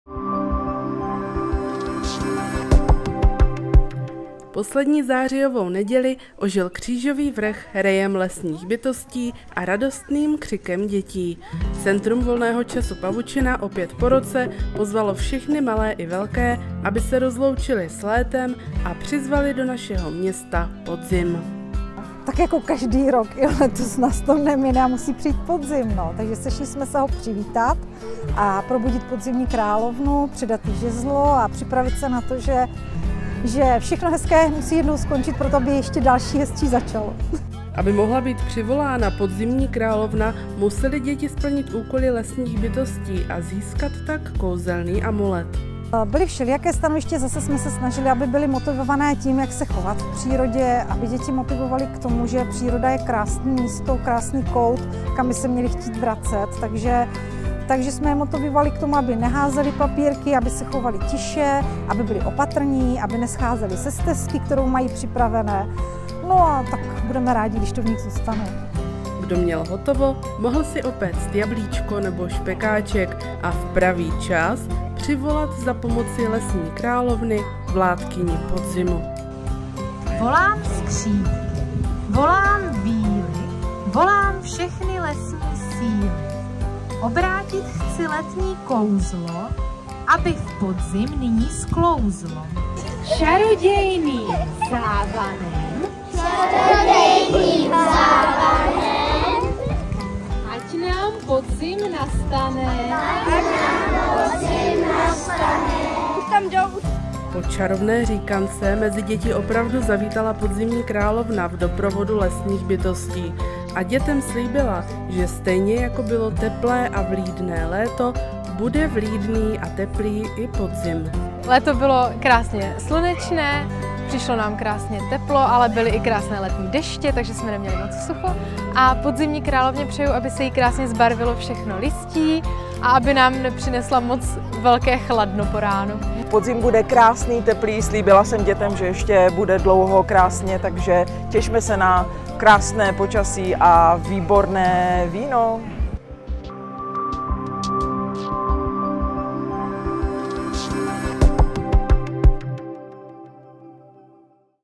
Vláda léta skončila. Děti přivolaly podzim.
Poslední zářijovou neděli ožil Křížový vrch rejem lesních bytostí a radostným křikem dětí. Centrum volného času Pavučina opět po roce pozval všechny malé i velké, aby se rozloučili s létem a přizvali do našeho města podzim.